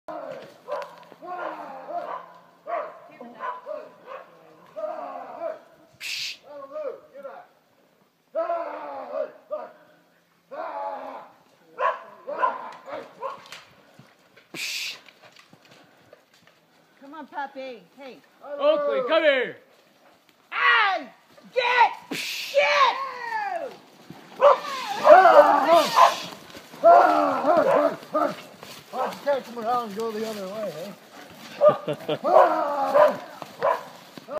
Bear encounter in Montana Forest
Horseback riders from Blacktail Ranch run into angry bear , cowboy Dan our guide and his dog Oakly kept the bear away from us til we got turned around and back the hill. I